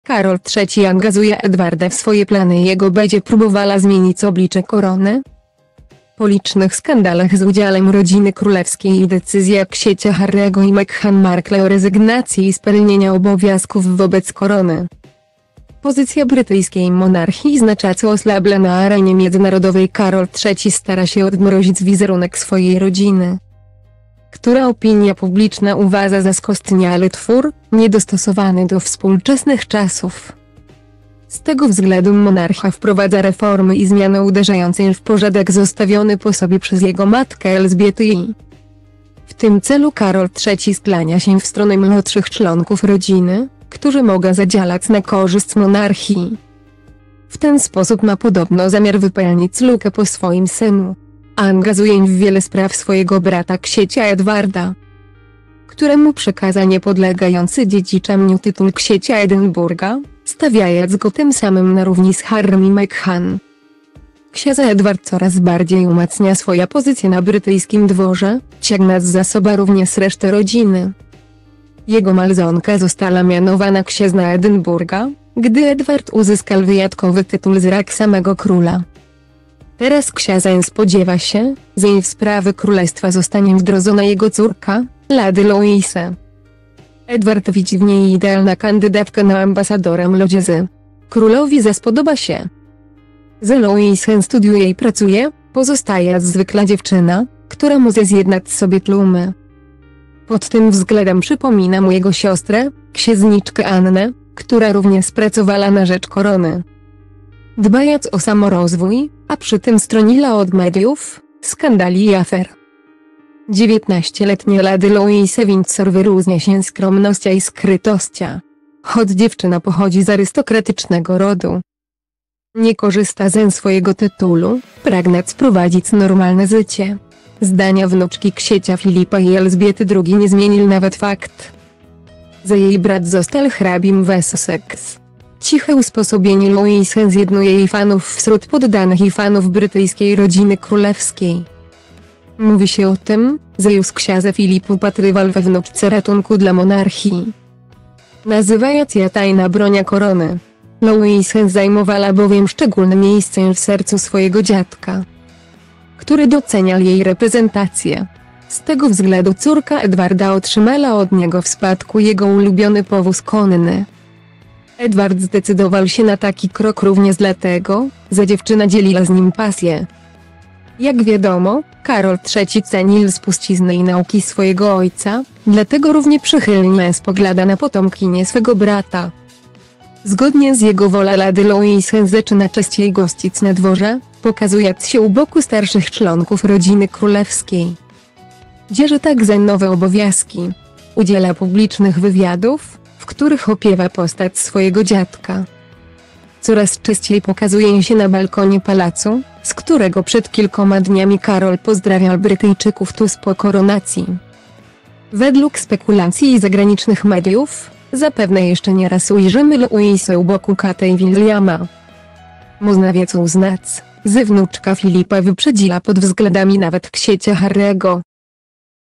Karol III angazuje Edwarda w swoje plany.Jego bedzie próbowala zmienic oblicze Korony?
Po licznych skandalach z udzialem rodziny królewskiej i decyzja ksiecia Harry’ego i Meghan Markle o rezygnacji z pelnienia obowiazków wobec Korony,pozycja brytyjskiej monarchii znaczaco oslabla na arenie miedzynarodowej.  Karol III stara sie odmrozic wizerunek swojej rodziny,która opinia publiczna uwaza za skostnialy twór, niedostosowany do wspólczesnych czasów.